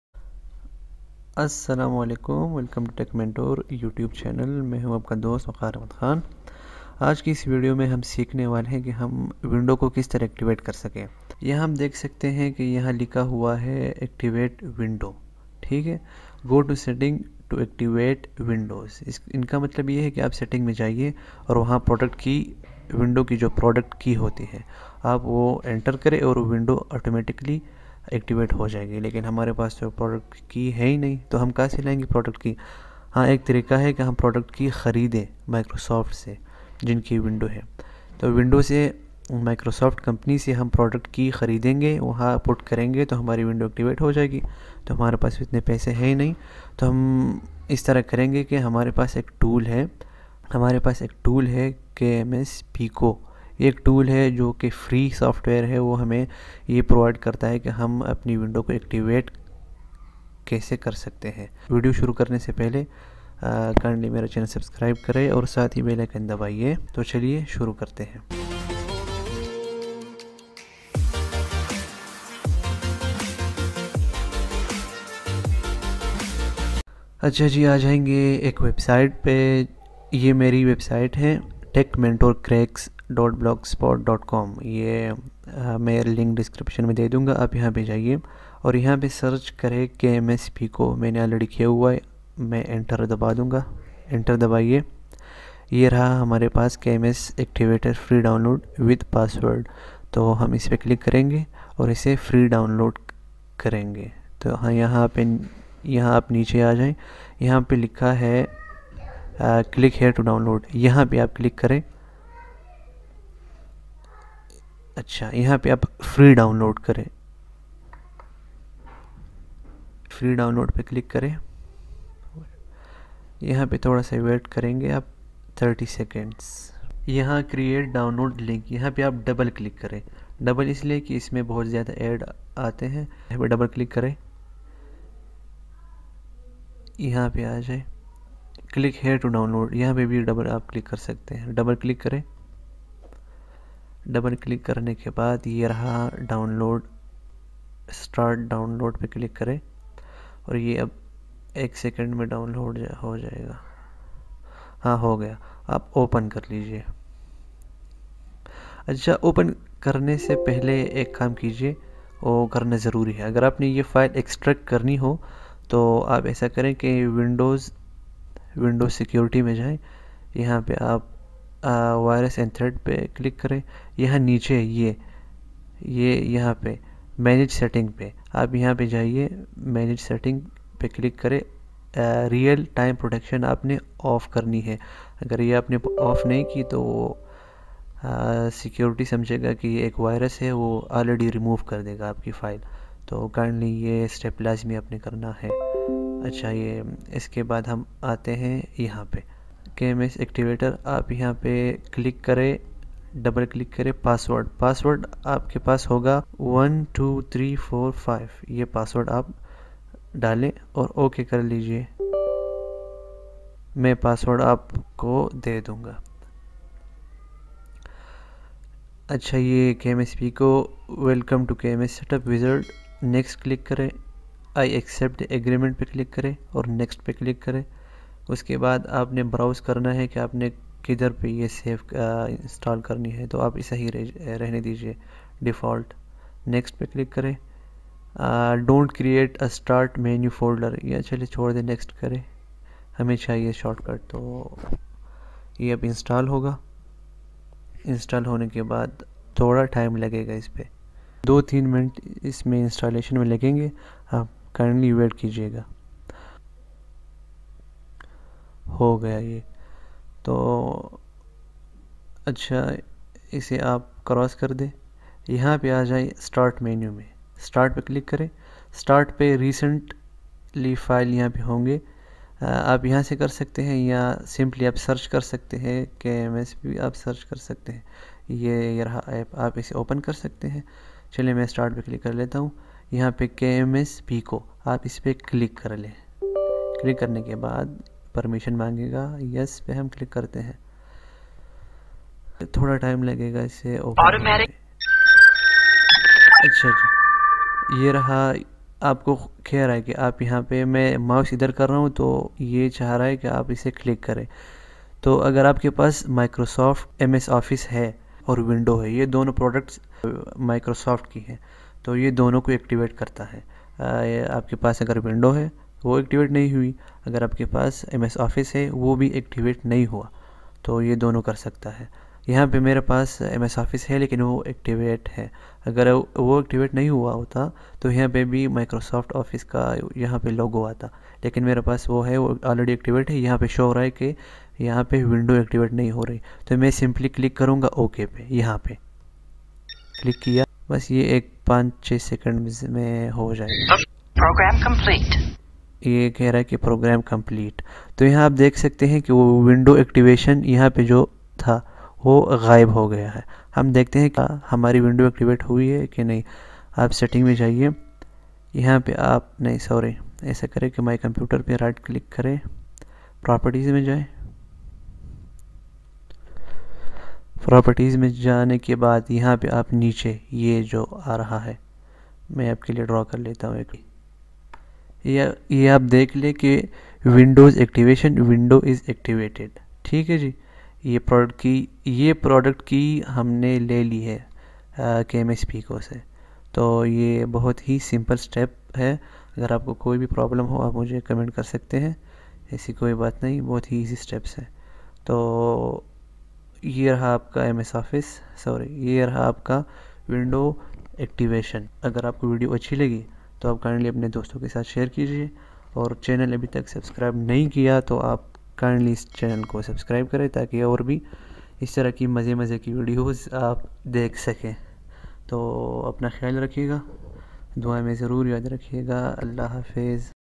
How To Activate Windows 10 In One Click | 100% Guarantee
Asslamam-O-Alaikum , Hello Welcome To Tech Mentor YouTube Channel
Do YOU want to learn How To Activate Windows 10 In One Click  so in this video I will show you how you can activate your windows 10/8/7 for free and permanent  so lets get started

Thank You Guys For Supporting Me